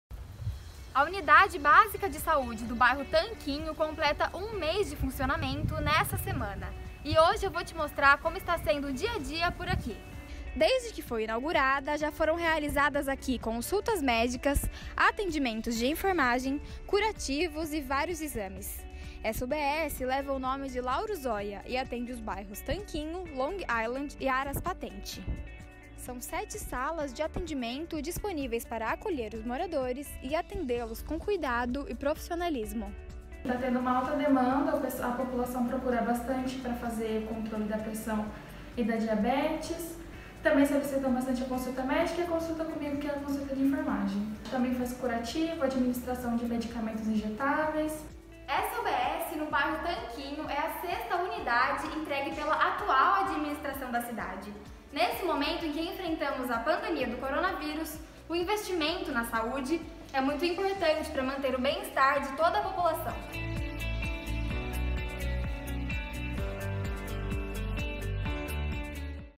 INVESTIMENTO NA SAÚDE
A UBS Tanquinho completa hoje o seu primeiro mês de funcionamento. Confira como está sendo o dia a dia dos atendimentos por lá!

Realização:
Prefeitura de Jaguariúna